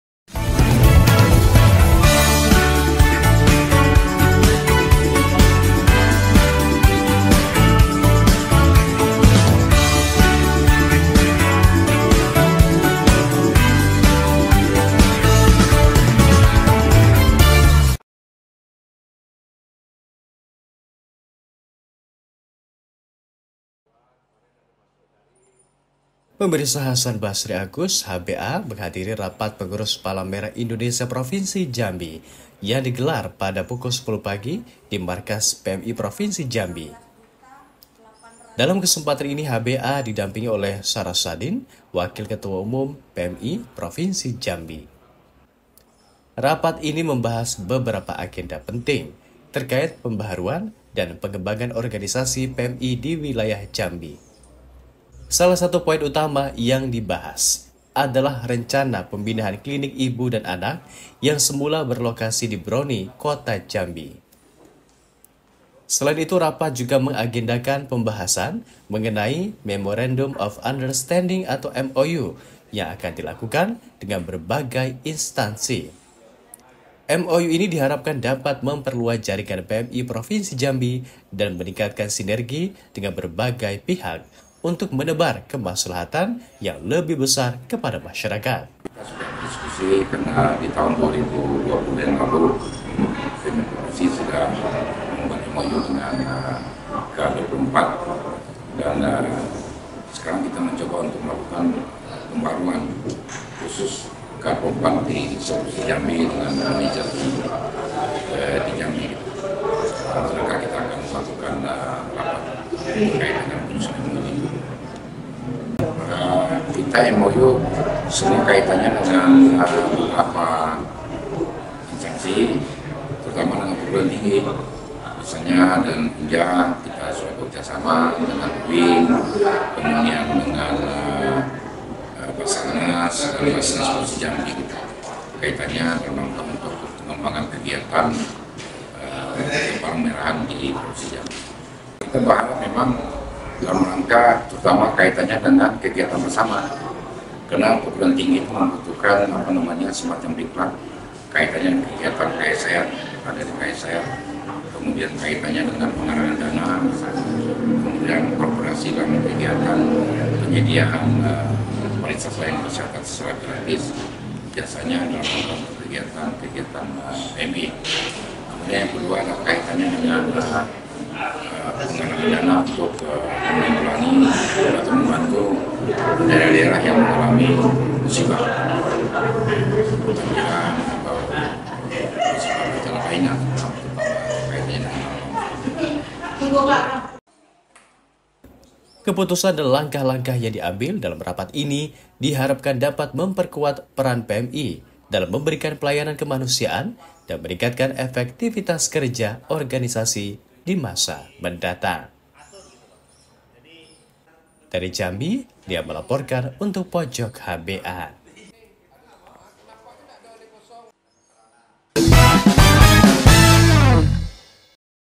HBA Gelar Rapat Pengurus PMI Provinsi Jambi, Ini yang Dibahas
JAMBI28TV, JAMBI - Pojok HBA episode ke 611 kali ini. Hasan Basri Agus (HBA) menghadiri rapat pengurus Palang Merah Indonesia (PMI) Provinsi Jambi yang digelar pada pukul 10 pagi di markas PMI Provinsi Jambi. Dalam kesempatan ini, HBA didampingi oleh Sarasadin, Wakil Ketua Umum PMI Provinsi Jambi.

Rapat ini membahas beberapa agenda penting terkait pembaharuan dan pengembangan organisasi PMI di wilayah Jambi. Salah satu poin utama yang dibahas adalah rencana pemindahan Klinik Ibu dan Anak yang semula berlokasi di Broni, Kota Jambi.

Selain itu, rapat juga mengagendakan pembahasan mengenai Memorandum of Understanding (MoU) yang akan dilakukan dengan berbagai instansi. MoU ini diharapkan dapat memperluas jaringan PMI Provinsi Jambi dan meningkatkan sinergi dengan berbagai pihak untuk menebar kemaslahatan yang lebih besar kepada masyarakat.

Keputusan dan langkah-langkah yang diambil dalam rapat ini diharapkan dapat memperkuat peran PMI dalam memberikan pelayanan kemanusiaan dan meningkatkan efektivitas kerja organisasi di masa mendatang.

Reporter: Lia